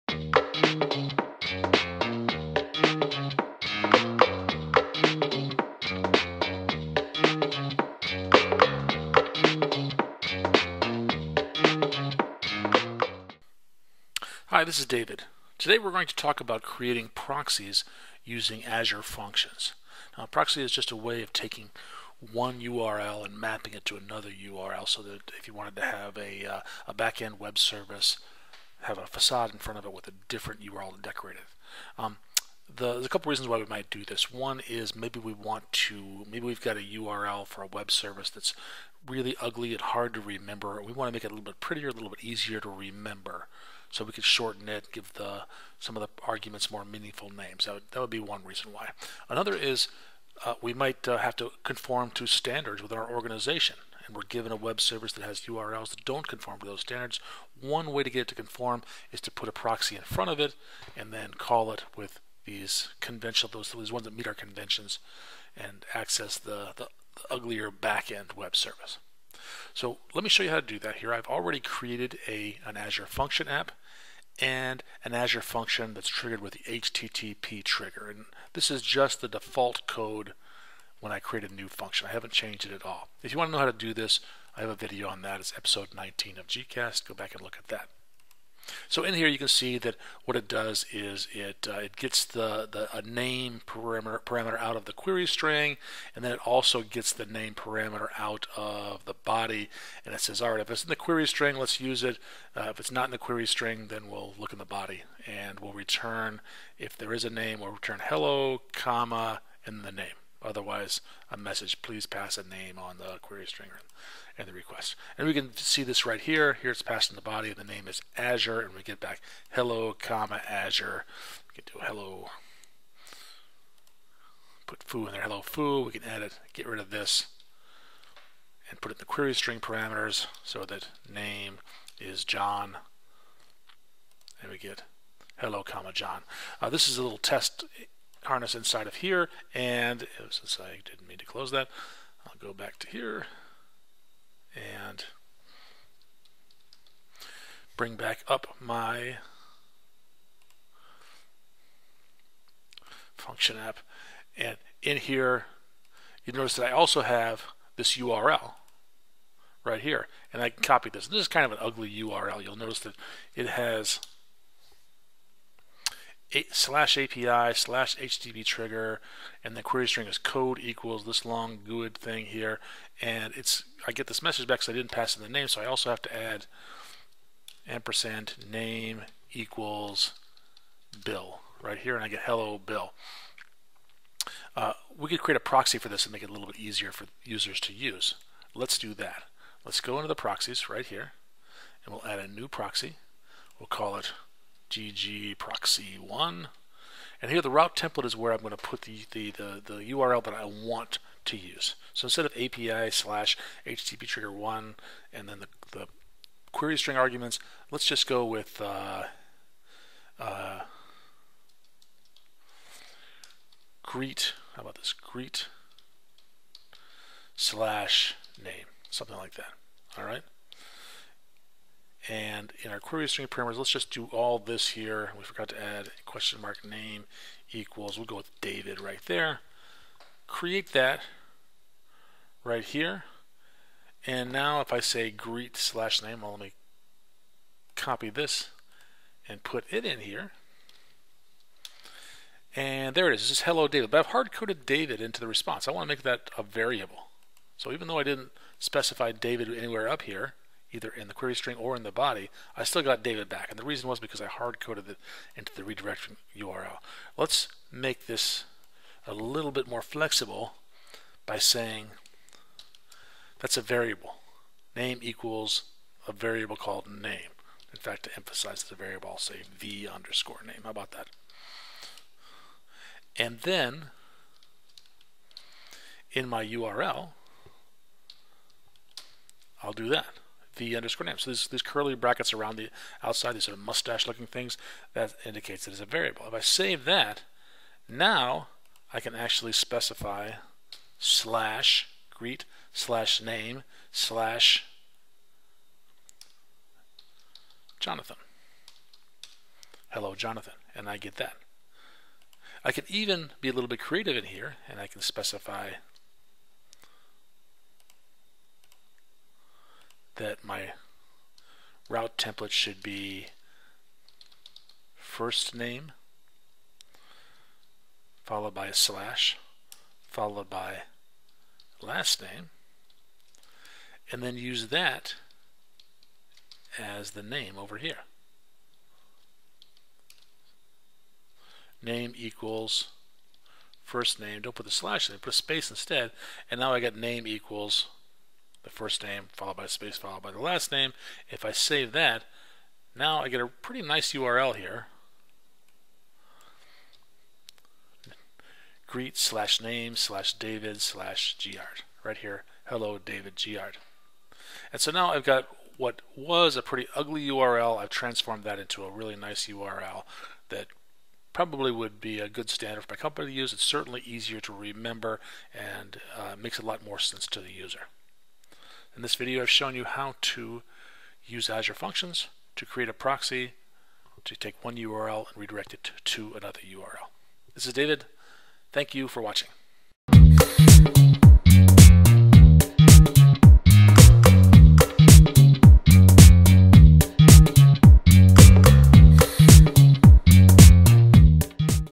GCast 22: Creating an Azure Function Proxy
Learn how to create a proxy URL using Azure Functions